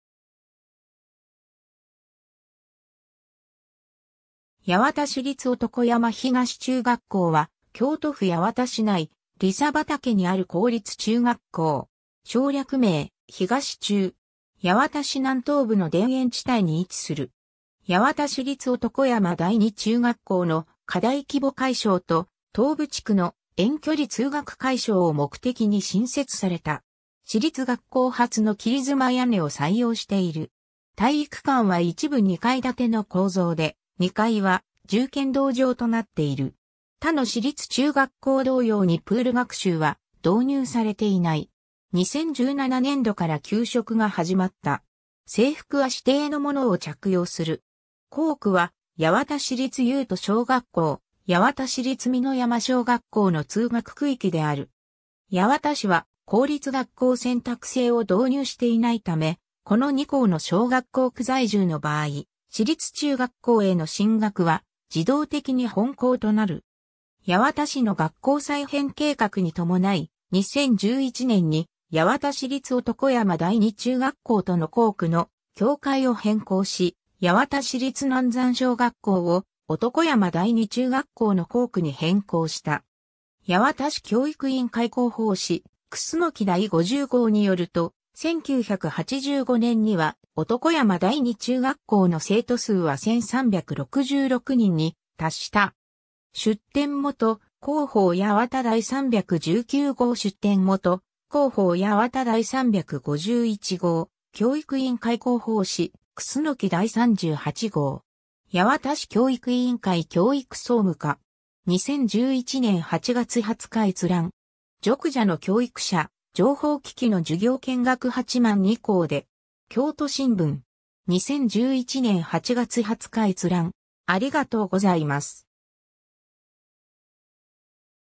八幡市立男山東中学校
八幡市立男山東中学校（やわたしりつ おとこやまひがしちゅうがっこう）は、京都府八幡市内里砂畠にある公立中学校。
省略名「東中」（ひがしちゅう・とんちゅう）。
 八幡市南東部の田園地帯に位置する。
八幡市立男山第二中学校の過大規模解消と、東部地区の遠距離通学解消を目的に新設された。
市立学校初の切妻屋根を採用している。
体育館は一部2階建ての構造で、2階は柔剣道場となっている。
他の市立中学校同様にプール学習は導入されていない。
 2017年度から給食が始まった。
 制服は指定のものを着用する。
 校区は八幡市立有都小学校、八幡市立美濃山小学校の通学区域である。
 八幡市は公立学校選択制を導入していないため、この2校の小学校区在住の場合、市立中学校への進学は自動的に本校となる。
 八幡市の学校再編計画に伴い、2011年に八幡市立男山第二中学校との校区の境界を変更し、八幡市立南山小学校を男山第二中学校の校区に変更した。
 ^ 八幡市教育委員会広報誌「くすのき」第50号（2006年2月28日発行）によると、1985年（男山東中学校分離前年）には男山第二中学校の生徒数は1366人に達した。
 ^ 出典元：「広報やわた」第319号（1990年4月発行） ^ 出典元：「広報やわた」第351号（1992年11月発行） ^ “教育委員会広報紙「くすのき」第38号”. 八幡市教育委員会教育総務課 (2004年7月1日). 2011年8月20日閲覧。
 ^ “ジョクジャの教育者、情報機器の授業見学　八幡2校で”. 京都新聞 (2010年10月29日). 2011年8月20日閲覧。